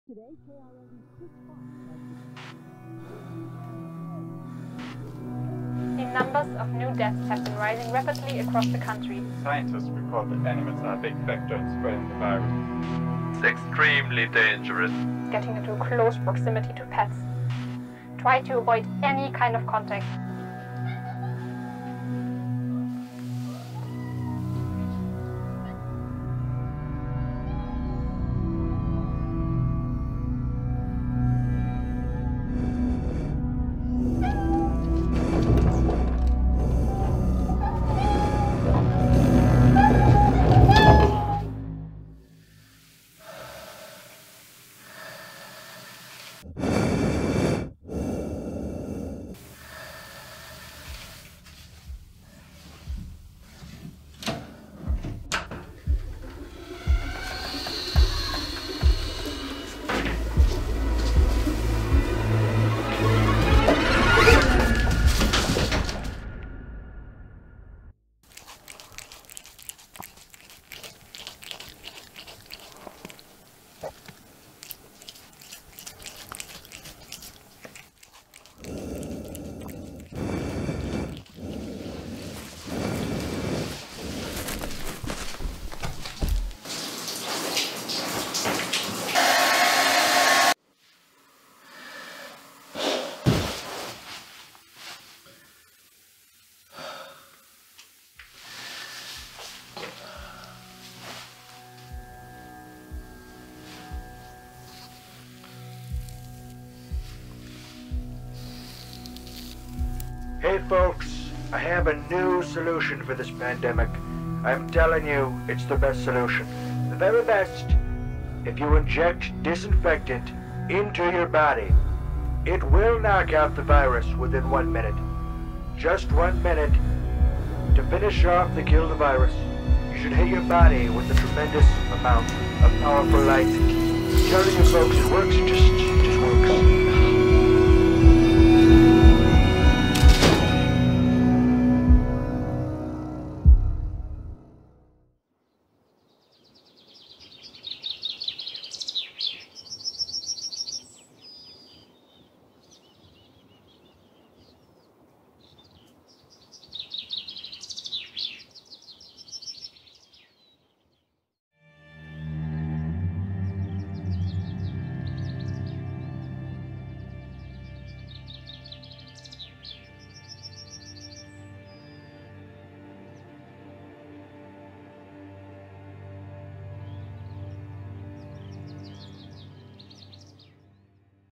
InFLUenced - Pandemic SHORT FILM
A Short Film about a paranoid Guy who went crazy based on the pandemic situation. Inspired by the COVID-19 happenings. Filmed with the BMPCC 4K.

produced by Frinds
directed by Victor Manske and Sven Lehmann